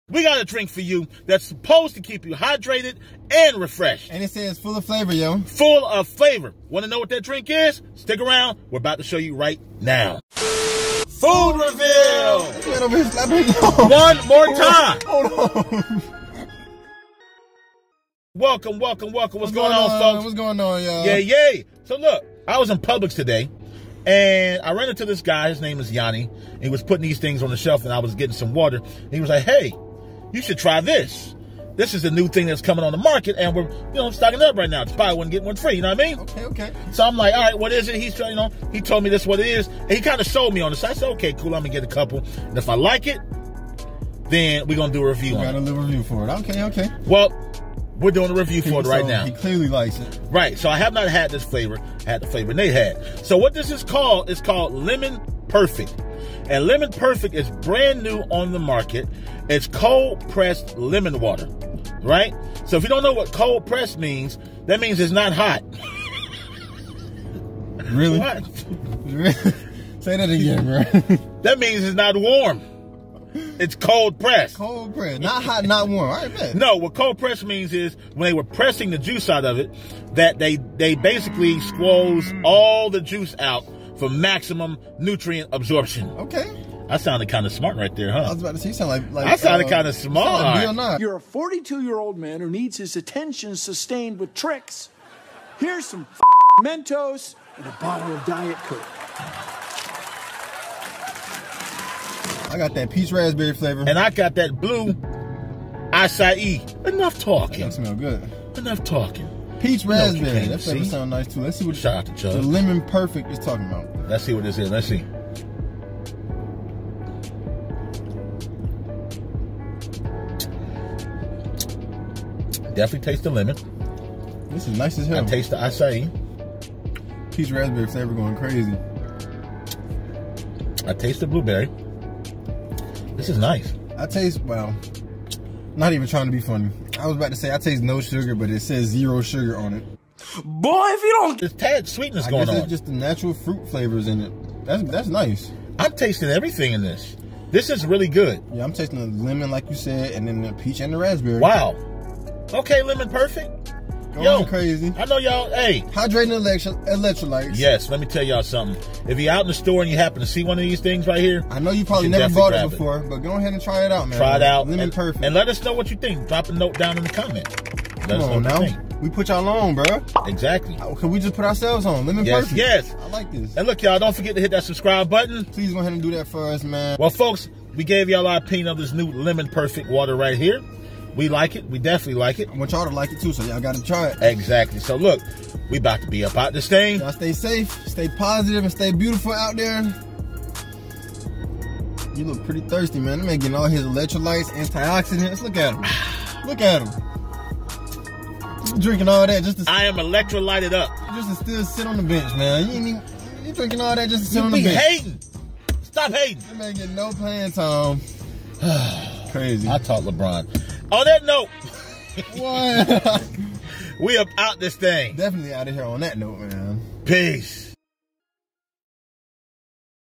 New Drink Alert! Lemon Perfect Cold Pressed Water
We just learned of a new hydrating drink that just dropped on the market called Lemon Perfect. It's a cold pressed water that is supposed to super hydrate you. Does it do the job? Lets find out together.